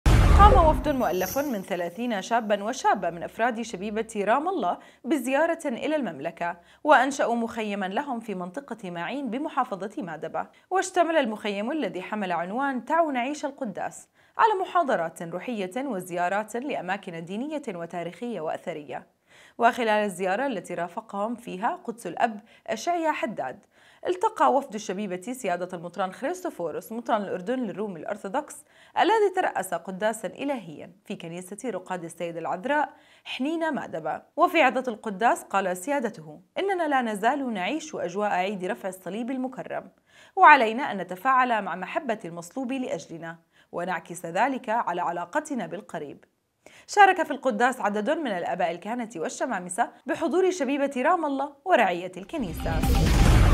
وفدٌ مؤلفٌ من ثلاثين شاباً وشابةً من أفرادِ شبيبةِ رام الله بزيارةٍ الى المملكه وأنشأوا مخيماً لهم
قام وفدٌ مؤلفٌ من ثلاثين شاباً وشابةً من أفرادِ شبيبةِ رام الله بزيارةٍ الى المملكه وأنشأوا مخيماً لهم في منطقةِ ماعين بمحافظةِ مادبا / واشتمل المخيمُ الذي حمل عنوان تعوا نعيشَ القداس على محاضراتٍ روحيةٍ وزياراتٍ لأماكنَ دينيةٍ وتاريخيةٍ وأثريه / وخلال الزياره التي رافقهم فيها قدسُ الأب إشعيا حداد / التقى وفدُ الشبيبةِ سيادةَ المطران خريستفورس مطرانَ الأردنِ للرومِ الأرثوذكس الذي ترأسَ قداساً الهياً في كنيسةِ رقادِ السيدةِ العذراء حنينا مادبا / وفي عظة القداس قالَ سيادَتُهُ إننا لا نزالُ نعيشُ أجواءَ عيدِ رفعِ الصّليبِ المكرم / وعلينا أن نتفاعلَ معَ محبّةِ المصلوبِ لأجلِنا ونعكِسَ ذلك على علاقتِنا بالقريب / شاركَ في القداسِ عددٌ من الآباءِ الكهنةِ والشمامسه بحضور شبيبةِ رام الله ورعيةِ الكنيسه